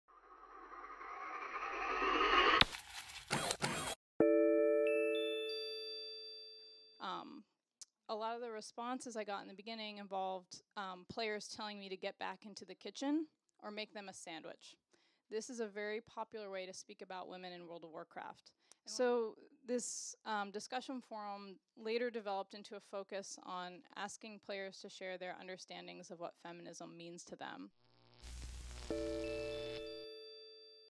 Angela Waskho
Intervention de Anglela Wasko. Conférence Hubertine est une Geek du 15 octobre 2015 "Cyber-activisme".